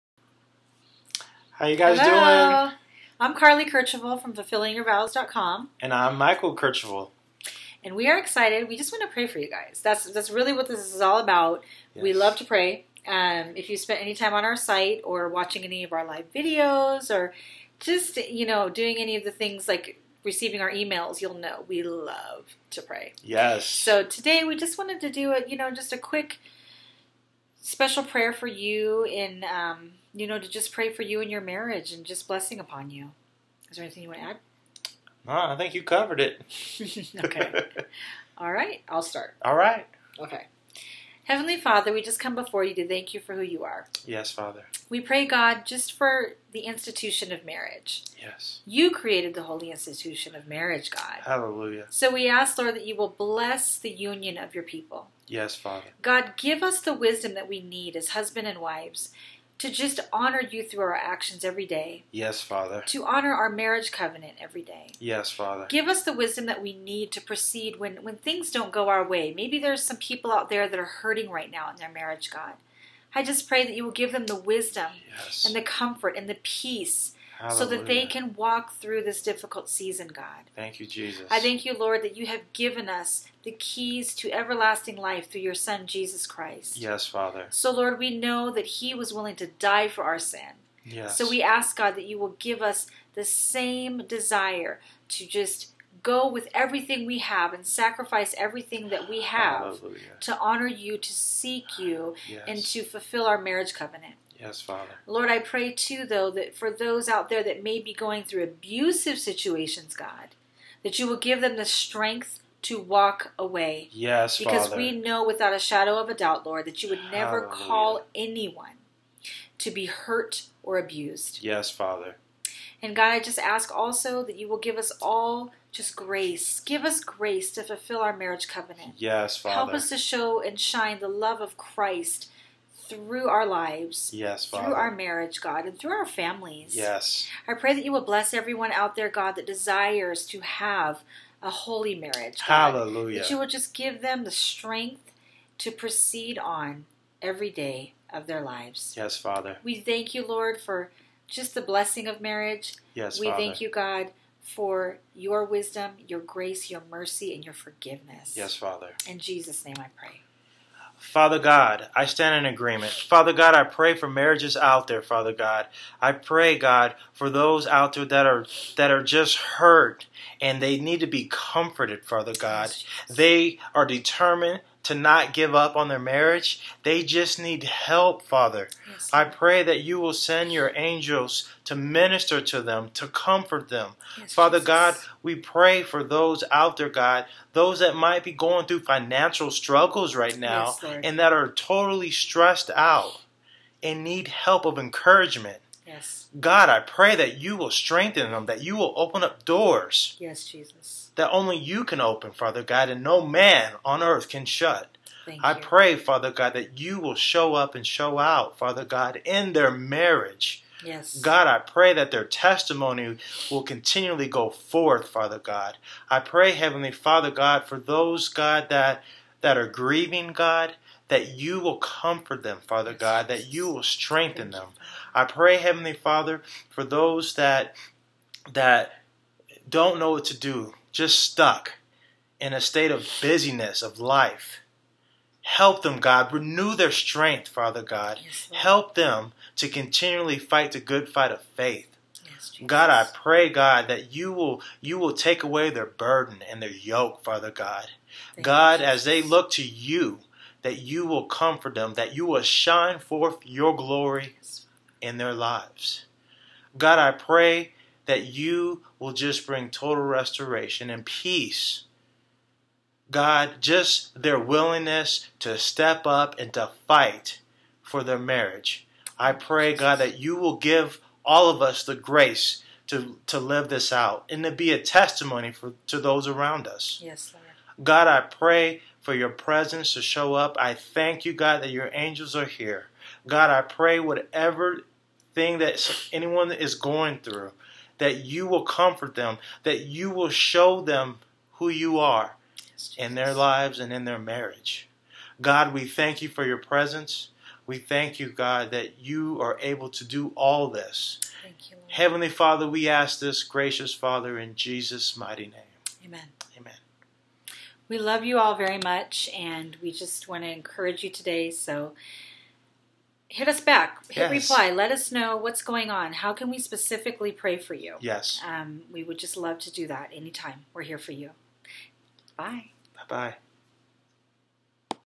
A Prayer for Your Marriage with Mike + Carlie Kercheval
Hello! Hello! Mike + Carlie Kercheval from FulfillingYourVows.com here :) We just wanted to pray for your marriage and let you know how much we love and appreciate each one of you. If you haven't already, we'd love for you to join our 5-week Prayer + Communication Challenge for Couples.